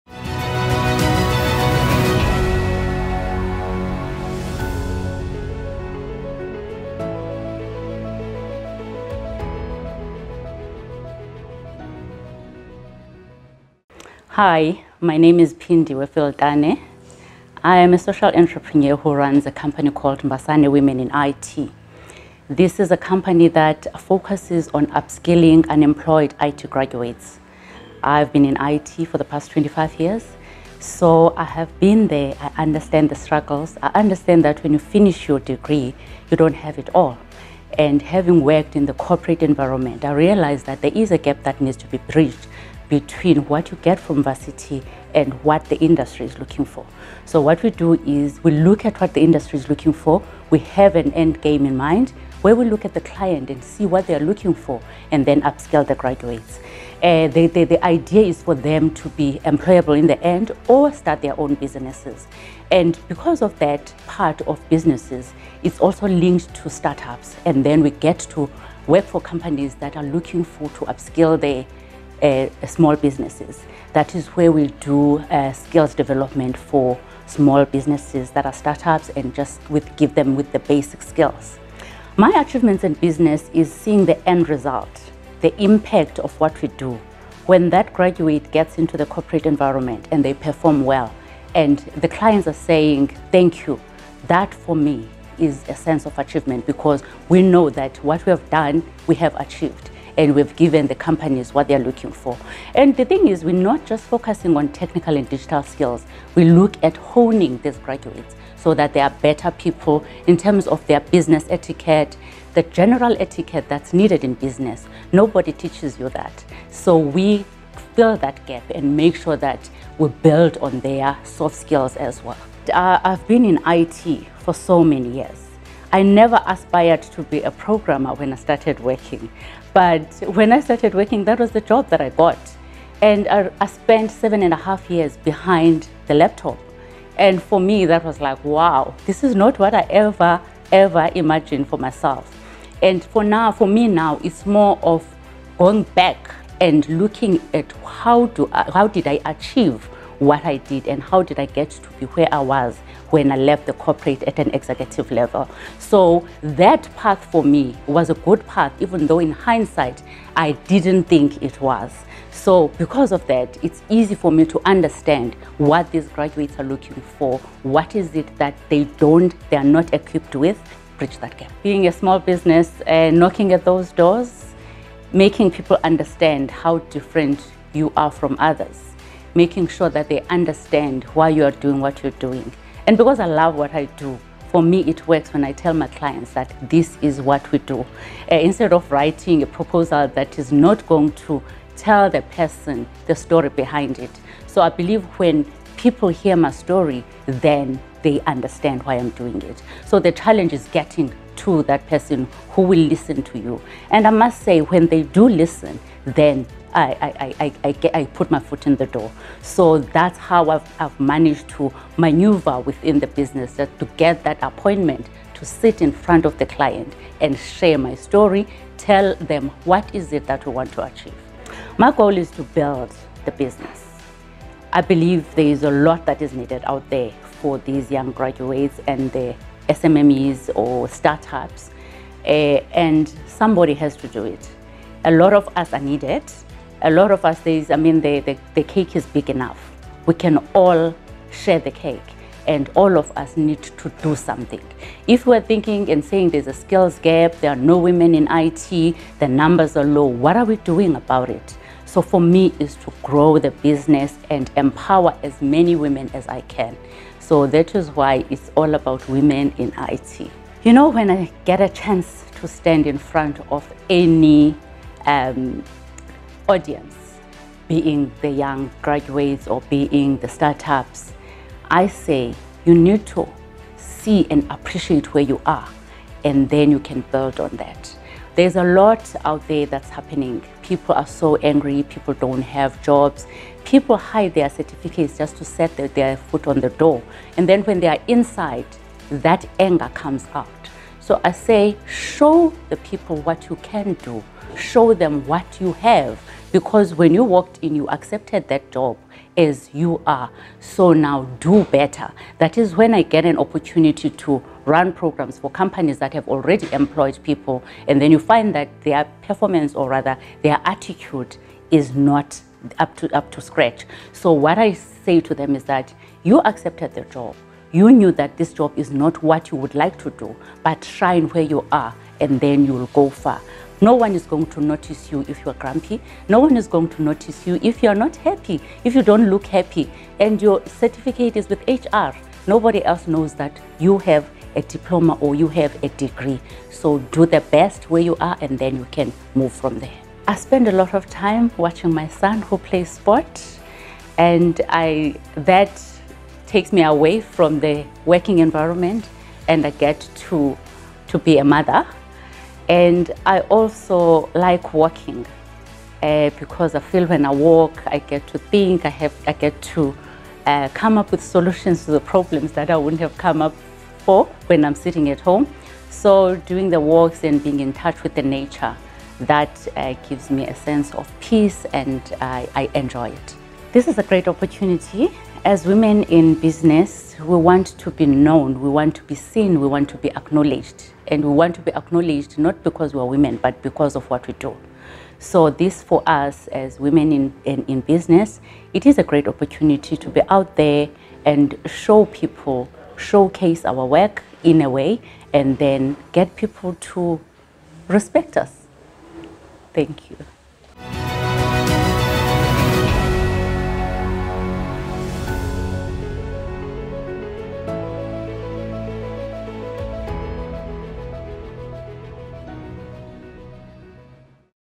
Pindiwe Filtane KZN Top Business Women 2022
PINDIWE FILTANE is a social entrepreneur who runs a company called Mbasane Women in IT. This is a company that focusses on upskilling unemployed IT graduates. She has been in IT for the past 25 years, so she says that she understands the struggles.